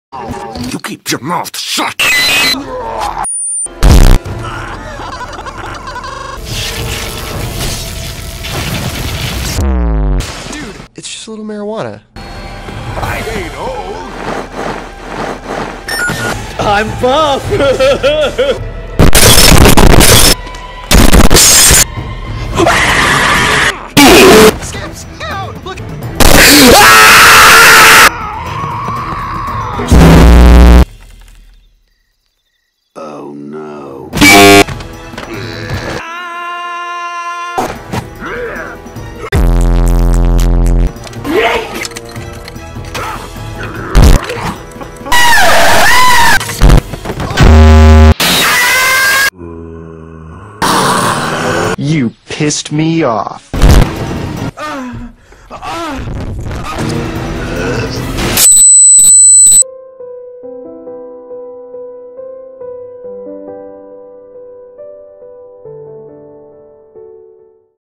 Movie Night Brawl (EARRAPE)
The boys (and a simp) take on a zombie apocalypse.